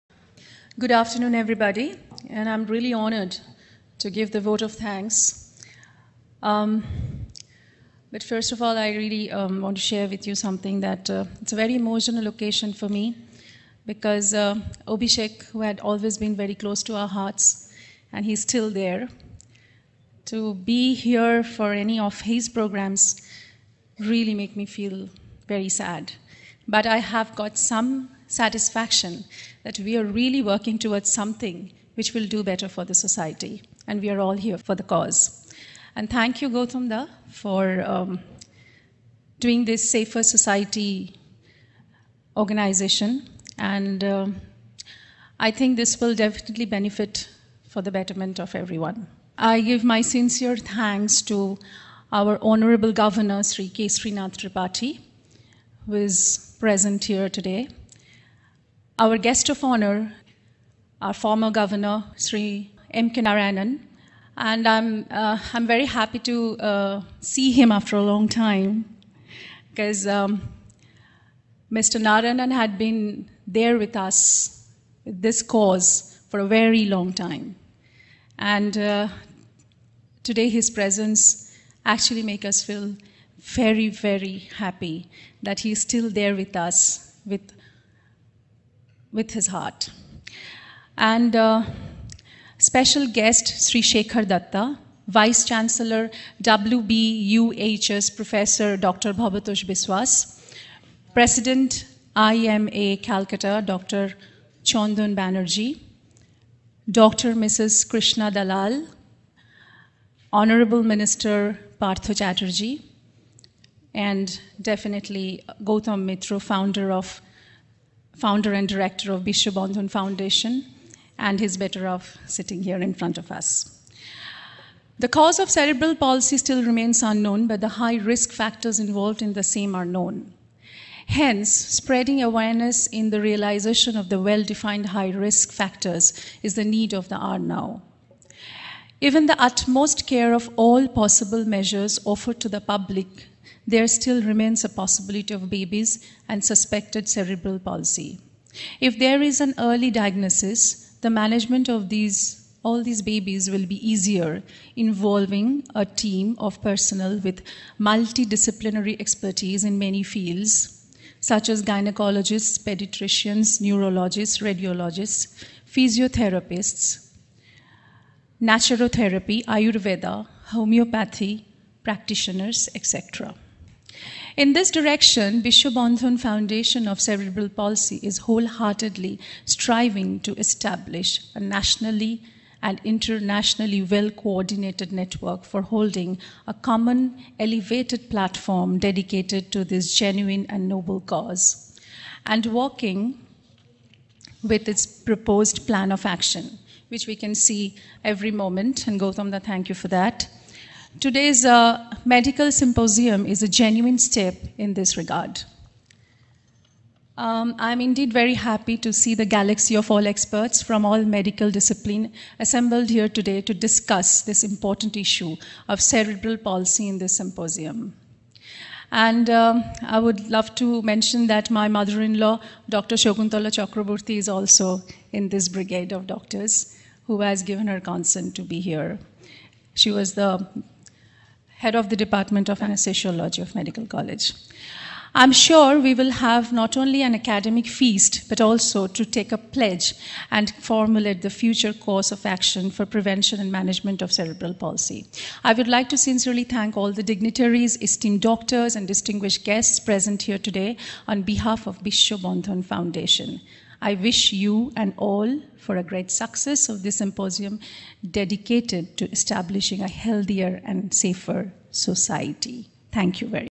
Rituparna Sengupta on Bishwabandhan Foundation India for Cerebral Palsy
Rituparna Sengupta on Bishwabandhan Foundation India for Cerebral Palsy and we are previldge for the Vote of Thanks on the Medical Symposium at Taj Bengal Kolkata, India